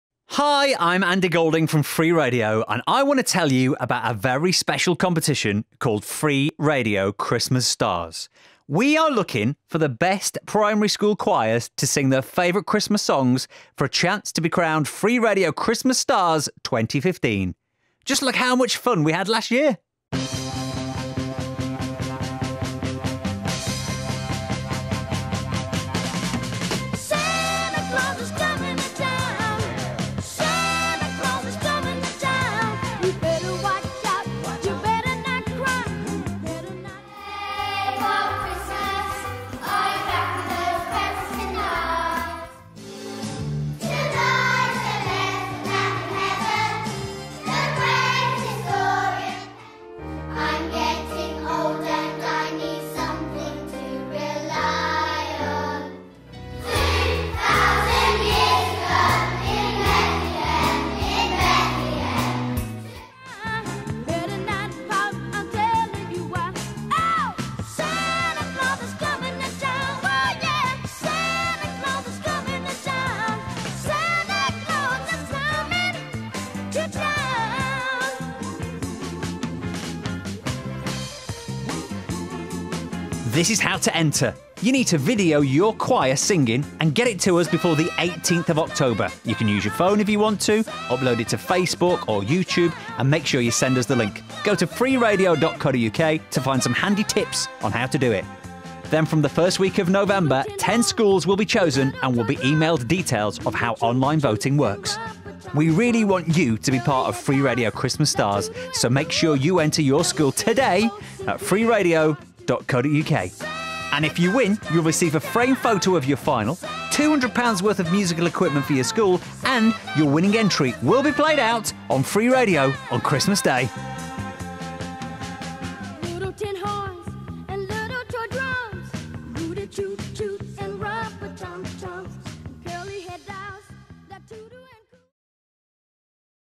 Christmas Stars - Enter our Primary School Choir Competition
Are you the next Free Radio Christmas Stars?

This festive season (it’ll be snowing before you know it!) we’re looking for primary school choirs to compete to become our Free Radio Christmas Stars.

Just film your school choir singing their festive song, upload it Youtube or Facebook and send us the link in the form below. Don't worry about your video being too fancy, as long as it sounds good and it's filmed landscape it's perfect.

You’ve got until 18th October to get your entries in, so get fine-tuning those Christmas jingles...